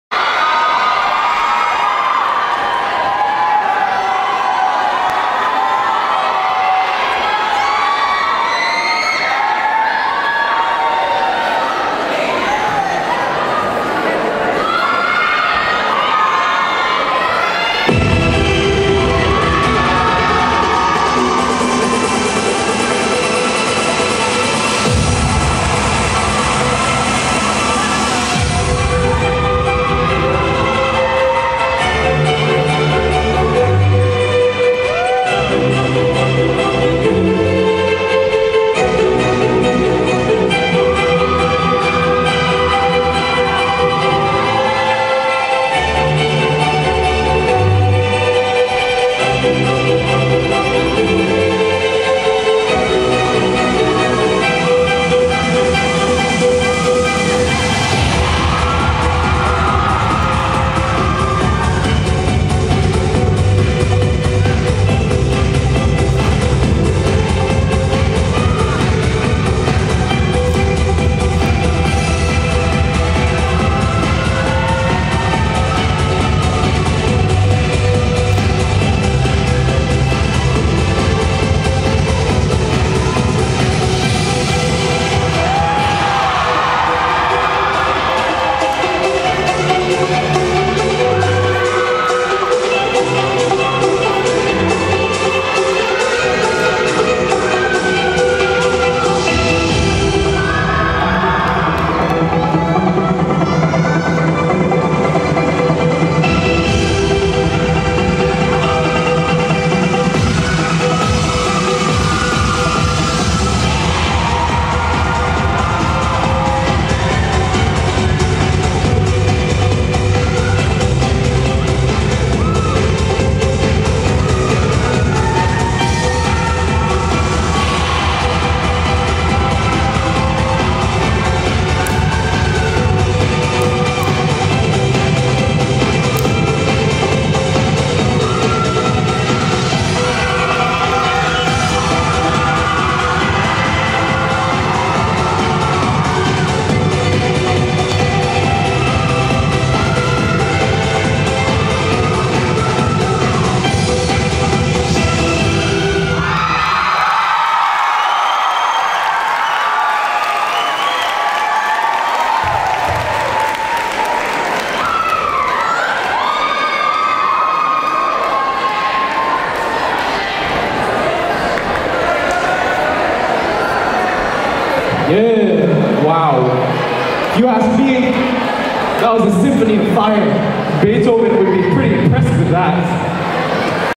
Shamrock 2015 - Notre Dame Junior (A)
Shamrock Dance Competition 2015, proudly presented by Saint Patrick Regional Secondary on Saturday,February 28, 2015.

Event Coordinators: Ms. Carlota Reyes & Ms. Tracy Nguyen